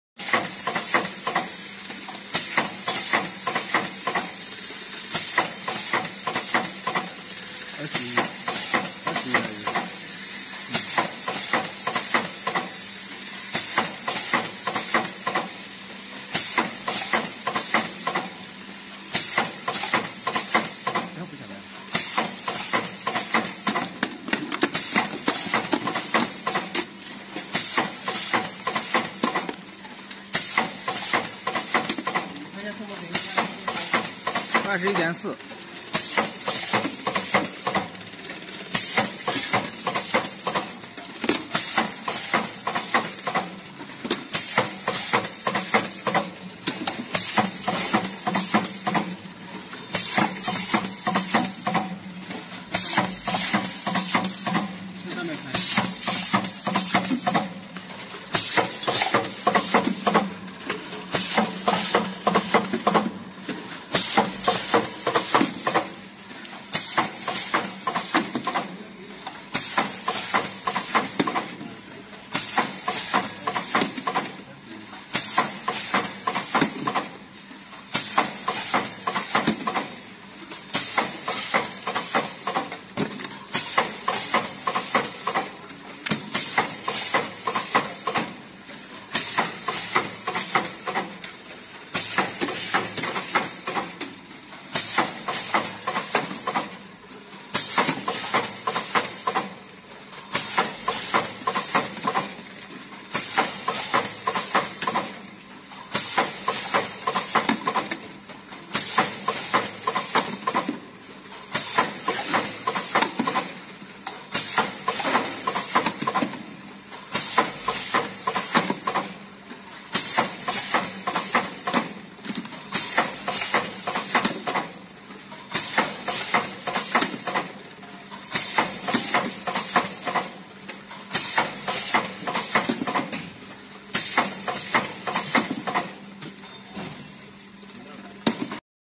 Rotary bottle filling machine
Rotary bottle filling machine, configure 3 pushers ensure products are totally inside the bottle. Solve the difficulty of the small bottle mouth diameter, but large produc size. Make sure smooth running in the further packaging process.